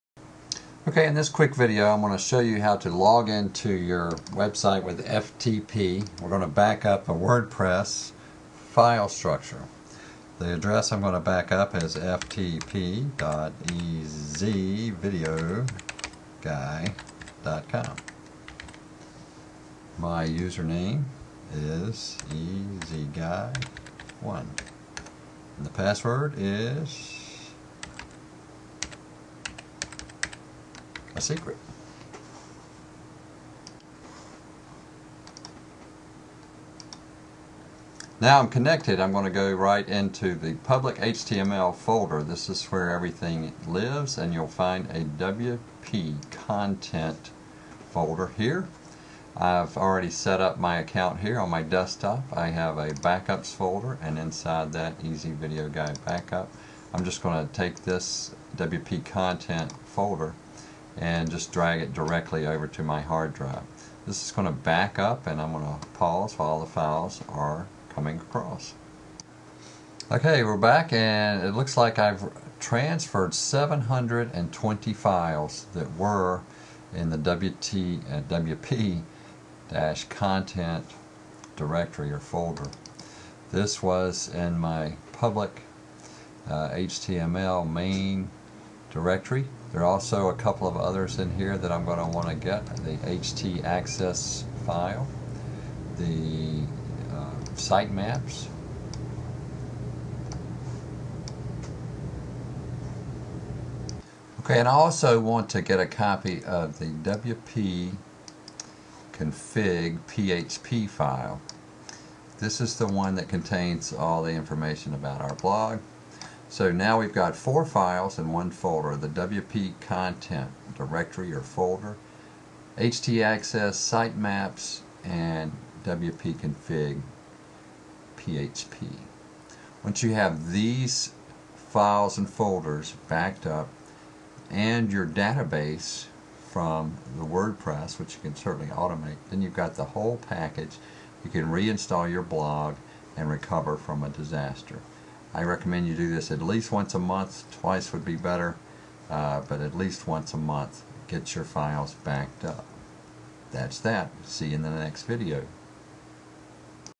backup your WordPress files w ftp
I use filezilla on a Mac & PC for accessing my host accounts. Here I am backing up my blog files to be used to recover if needed.